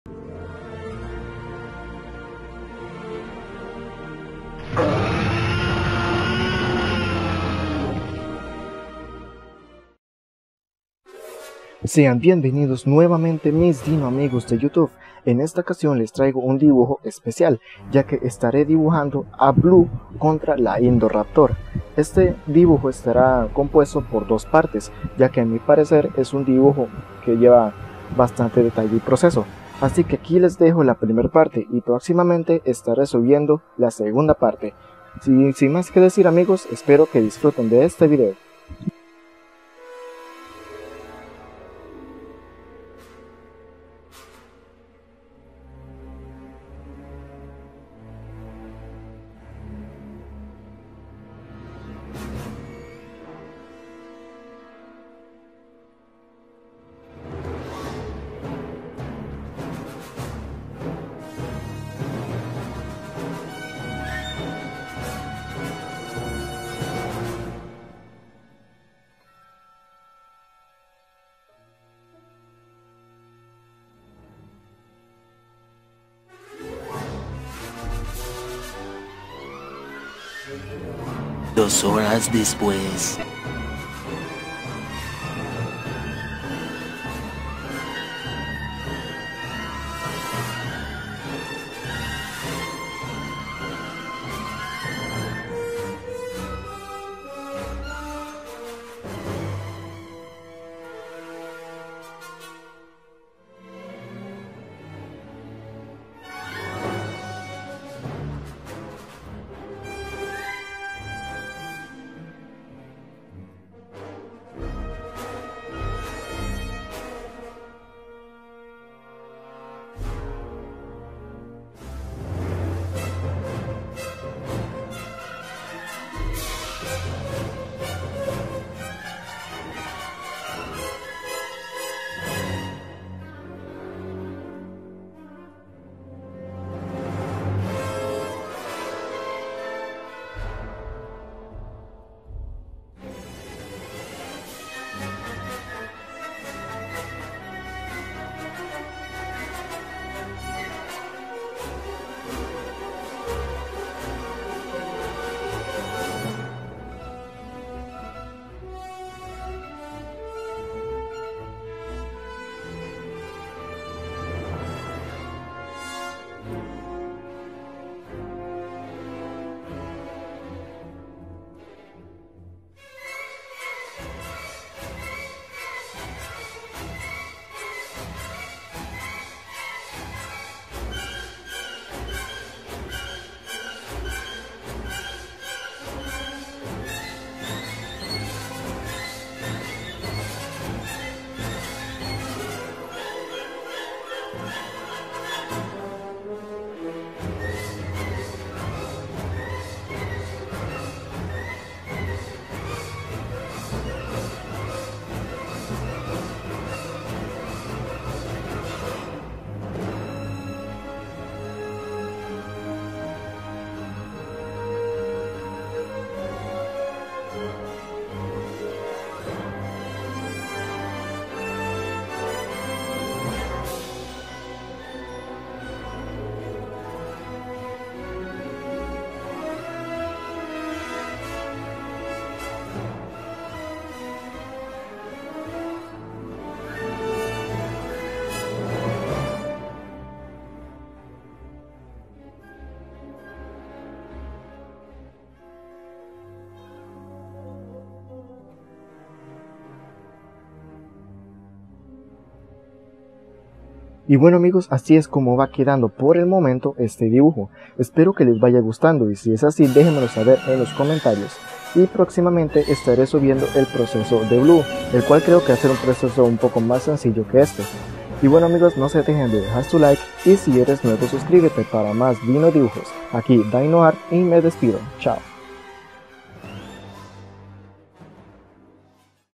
Especial || Blue vs Indoraptor Parte 1
Sean bienvenidos nuevamente mis dino-amigos :3
En esta ocasión les traigo un especial en proceso de blue vs el indoraptor algo que nosotros ya queremos ver en la pelicula :D
Próximamente estare subiendo la segunda parte de este dibujo y espero que les valla gustando tanto como a mi :3